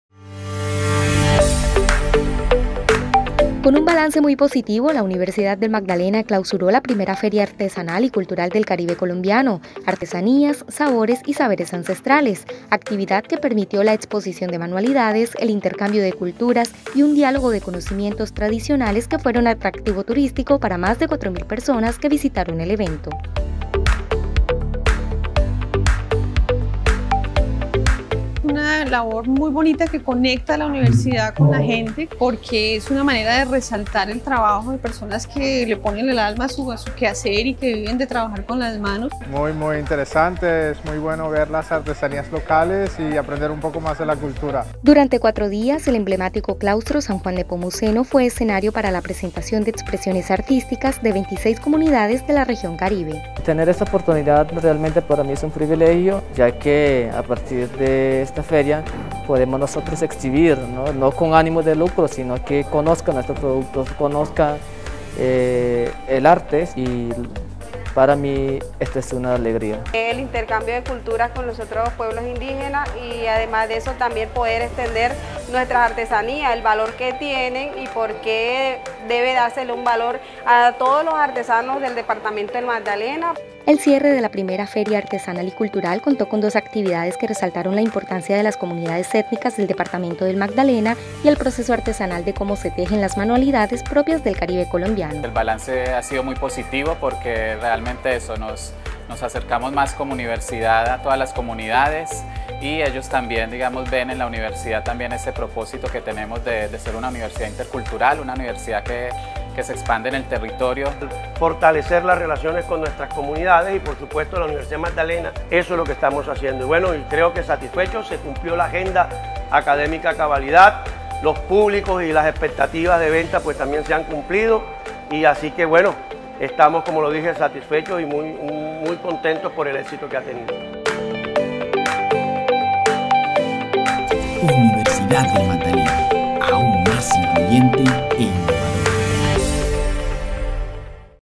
Culminó I Feria Artesanal y Cultural de UNIMAGDALENA
El acontecimiento que reunió a 12 etnias indígenas y comunidades diversas de la Región Caribe,  cerró con espacios académicos que mostraron la importancia de los tejidos y las mochilas, con el propósito de rescatar la importancia de las artesanías y los saberes ancestrales.